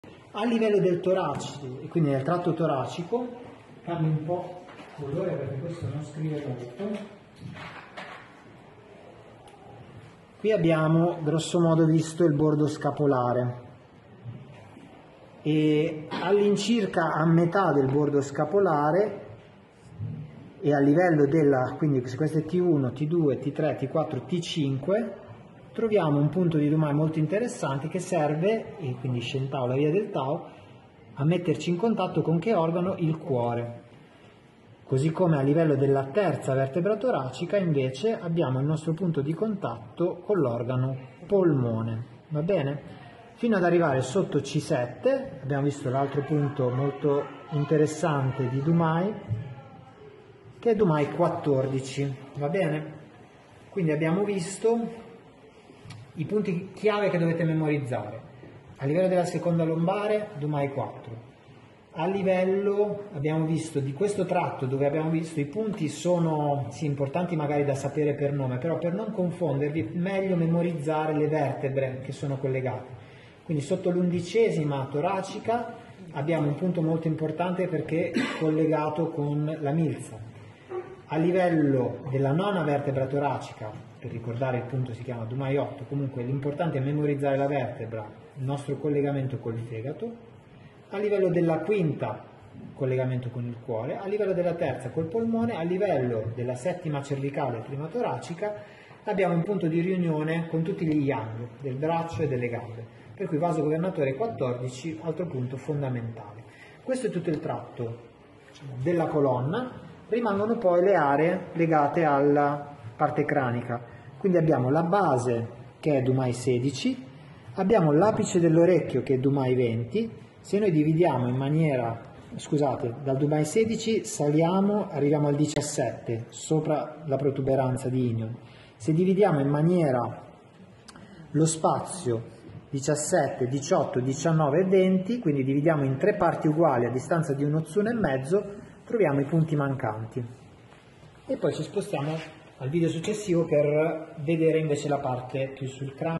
Localizzazione Dumai e Beishu (2) Punti e tecniche di Agopuntura
LOCALIZZAZIONE DUMAI E BEISHU - PARTE 2

Il canale di Dumai costituisce il meridiano straordinario di prima generazione di riferimento per la localizzazione di tutti i punti della zona dorsale del corpo, in particolare quelli del  canale di Vescica urinaria.
Essendo in connessione diretta con la colonna vertebrale, il cranio e il sistema nervoso centrale  non sorprende che sia in grado di esercitare una funzione di "governo" anche sugli organi interni. In questi due video esamineremo i reperi anatomici principali per la localizzazione dei punti classici di Dumai  e le corrispondenze esatte fra questi,  i punti beishu di vescica urinaria e i principali organi e visceri ( zangfu) .

I video di questa rassegna sono realizzati dal dr.Filippo Castrovilli docente del corso di Agopuntura a ScuolaTao e referente di StudioTao , via di Corticella 89 Bologna